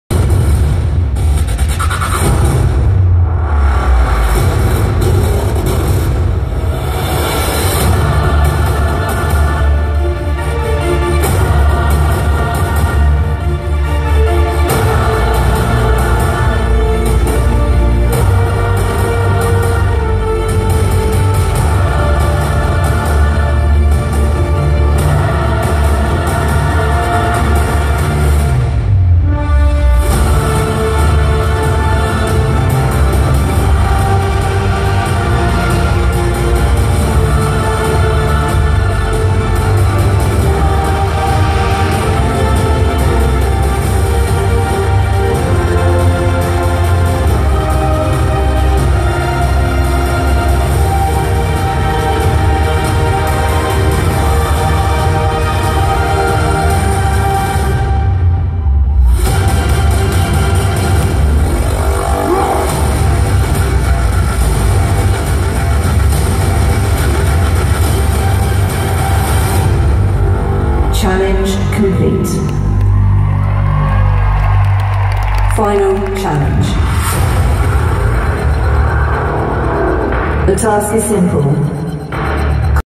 Diversity Genesis - part 6 - the Simulator
Genesis - Diversity 2017 Tour
Created by Ashley Banjo

Ashley tests out his power absorbtion during a challenge in the simulator.

The O2 Arena, London
14/04/17